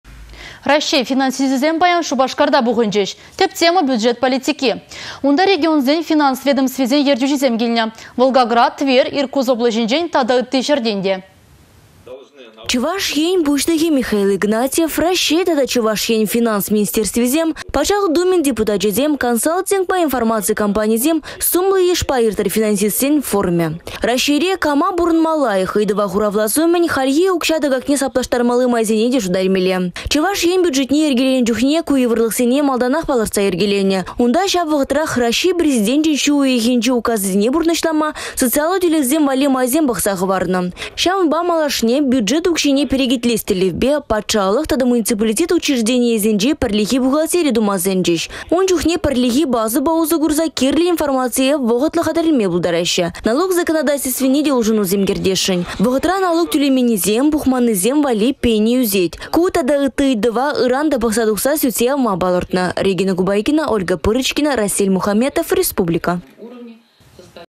Раççей финансисчĕсем — Чăваш Енре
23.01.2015
Раççей финансисчĕсем паян Шупашкарта пухăнчĕç. Тĕп тема — бюджет политики. Унта регионсен финанс ведомствисен ертÿçисем килнĕ: Волгоград, Тверь, Иркутск облаçĕсенчен тата ытти çĕртен те.